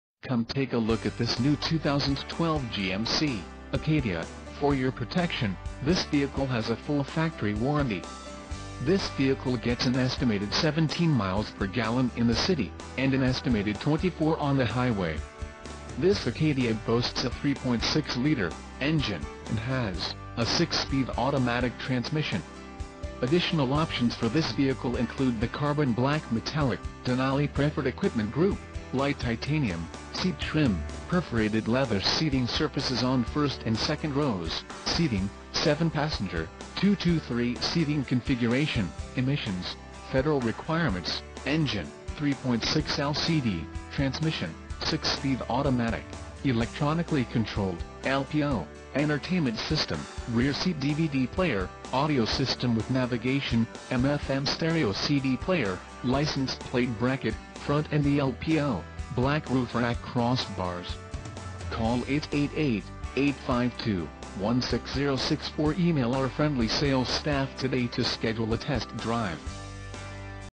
2012 GMC Acadia FWD 4dr Denali SUV - Columbia, SC
Jim Hudson Buick GMC Cadillac, Columbia, SC - Loaded! Leather, DVD, Nav, Dual Sunroof, Power Liftgate, Rear Camera, USB, More!